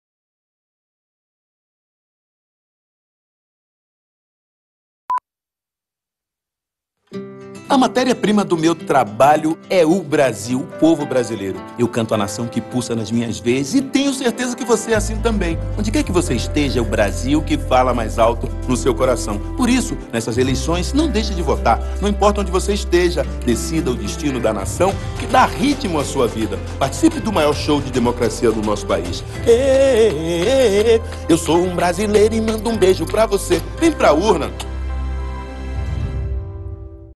Campanha eleitor no exterior Carlinhos Brown
Campanha da Justiça Eleitoral para esclarecer eleitor que mora no exterior.